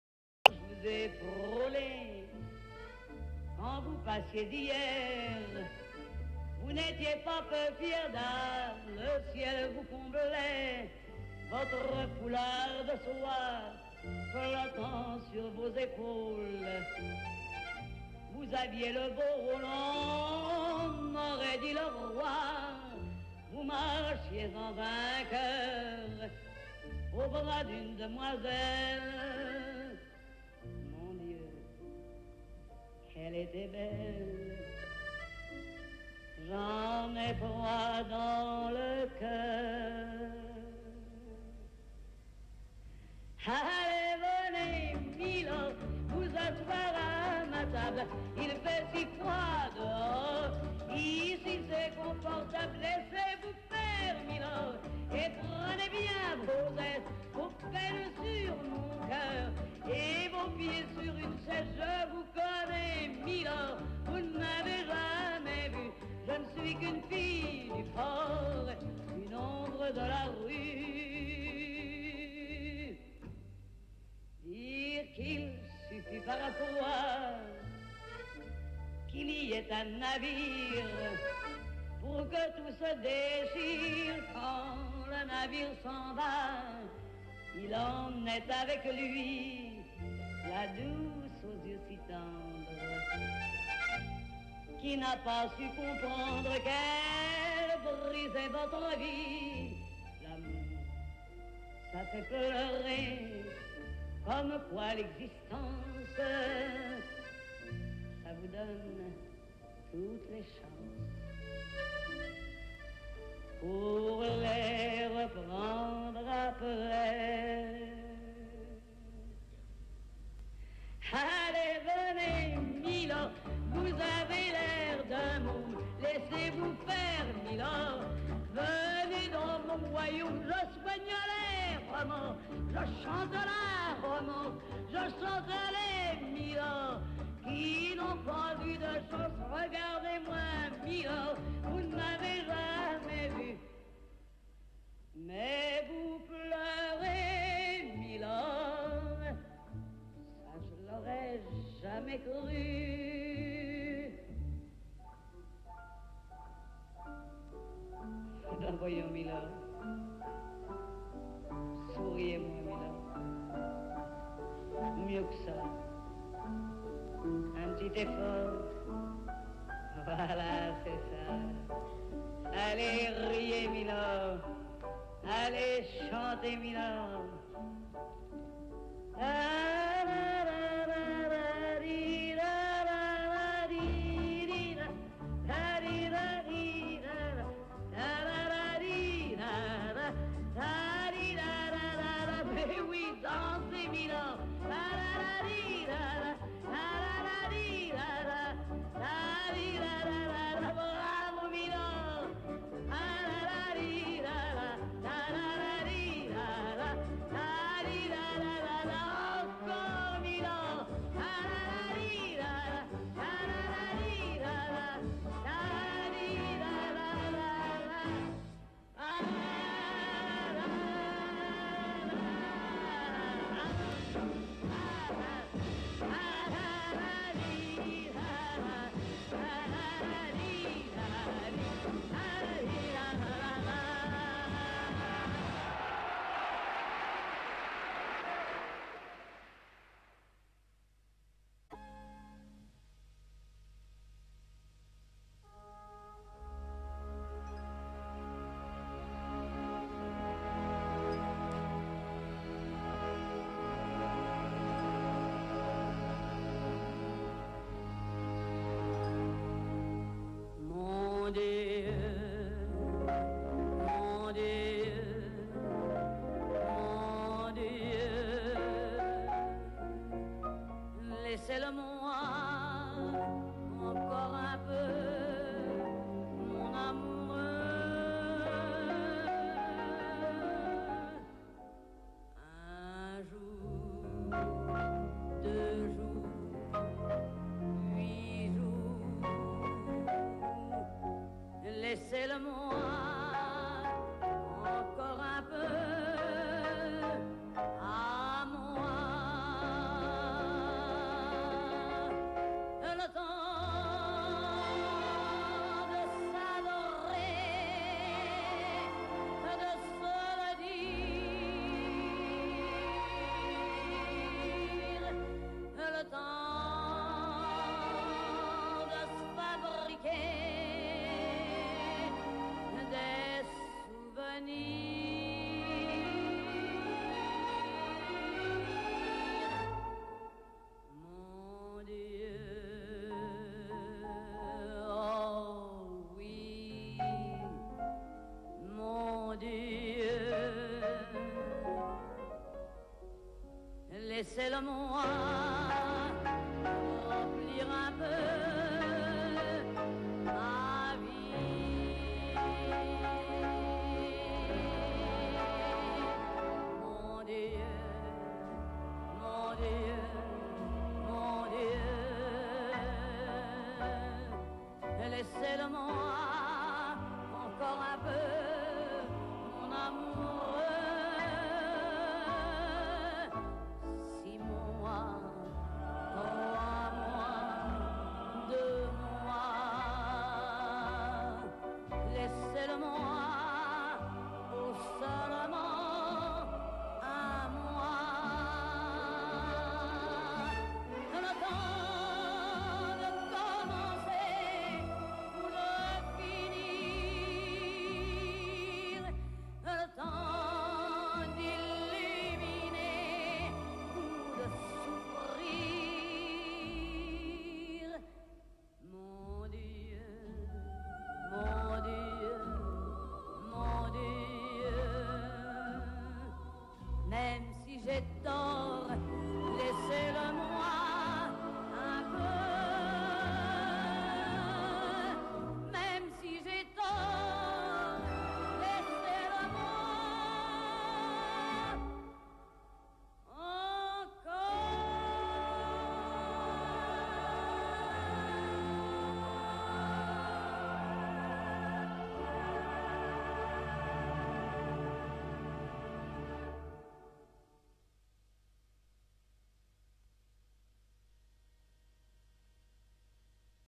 "Memory of Piaf" solo and duet (long ver.) choreographed by Vasco Wellenkamp (1987)
Performed by Gagik Ismailian and Graça Barroso.